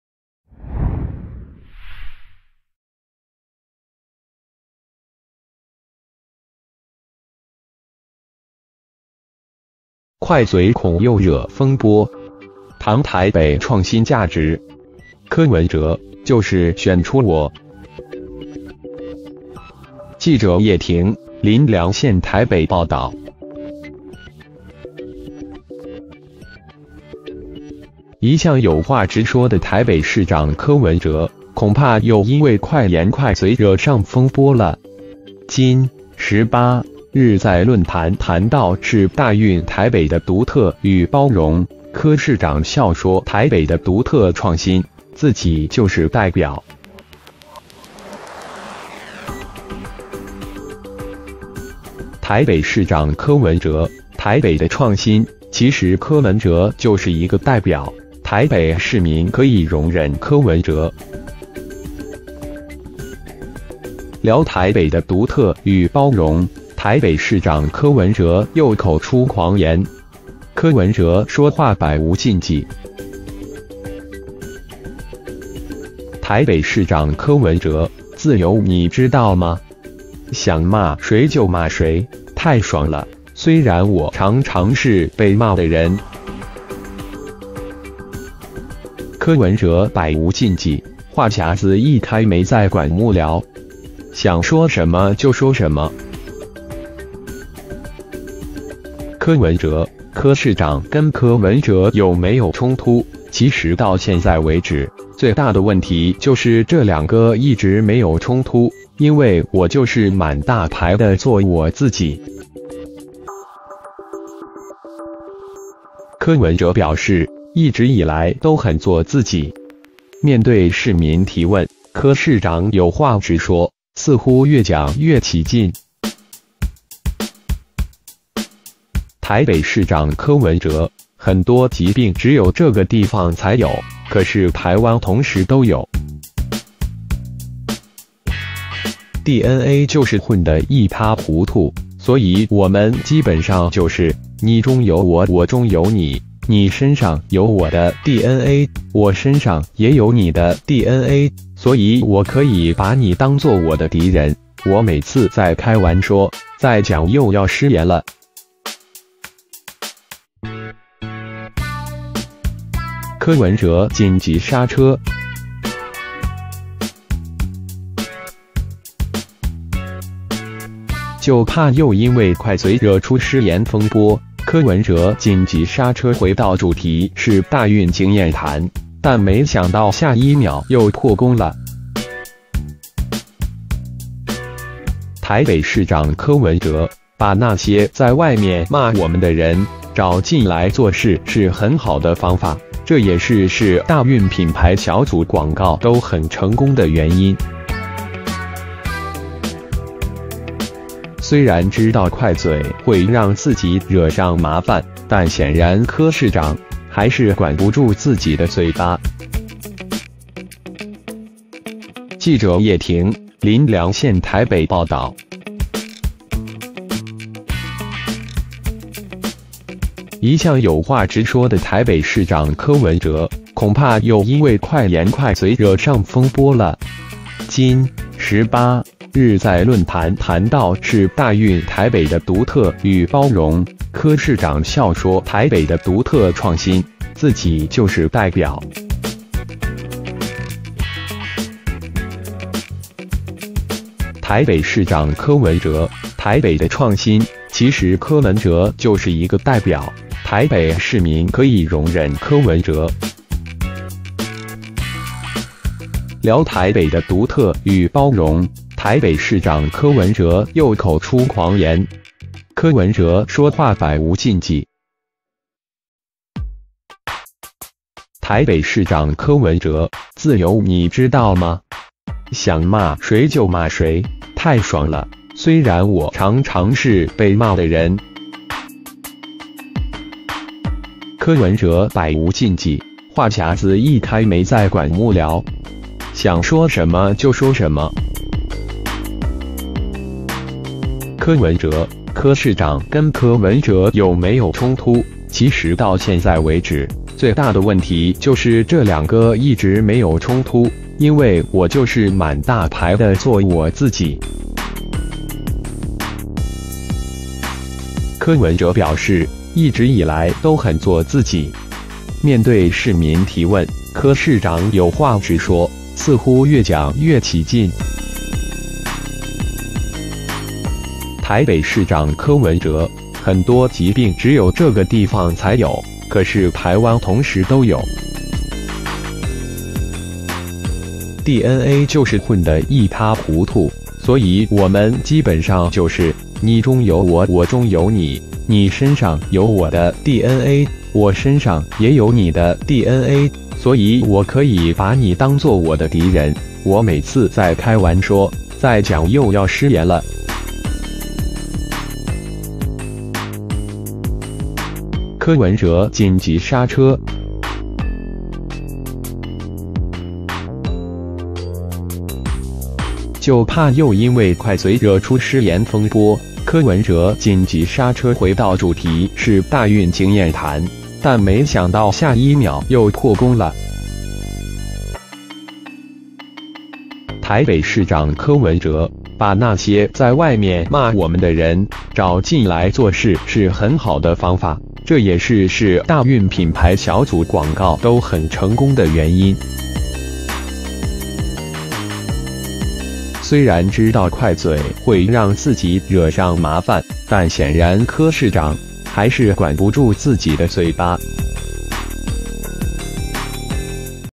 快嘴恐又惹風波！談台北創新價值 柯文哲：就是「選出我」
記者葉庭、林良憲／台北報導

一向有話直說的台北市長柯文哲，恐怕又因為快言快嘴惹上風波了！今（18）日在論壇談到「世大運台北的獨特與包容」，柯市長笑說台北的獨特創新，自己就是代表！

台北市長柯文哲：「台北的創新，其實柯文哲就是一個代表，台北市民可以容忍柯文哲。」聊台北的獨特與包容，台北市長柯文哲又口出狂言。

ID-1286901

▲柯文哲說話百無禁忌。

台北市長柯文哲：「自由你知道嗎？想罵誰就罵誰，太爽了，雖然我常常是被罵的人。」柯文哲百無禁忌，話匣子一開沒在管幕僚，想說什麼就說什麼。柯文哲：「柯市長跟柯文哲有沒有衝突，其實到現在為止，最大的問題就是這兩個一直沒有衝突，因為我就是滿大牌的做我自己。」

ID-1286899

▲柯文哲表示，一直以來都很做自己。

面對市民提問，柯市長有話直說，似乎越講越起勁。台北市長柯文哲：「很多疾病只有這個地方才有，可是台灣同時都有，DNA就是混得一塌糊塗，所以我們基本上就是，你中有我我中有你，你身上有我的DNA，我身上也有你的DNA，所以我可以把你當作我的敵人，我每次在開玩說，再講又要失言了。」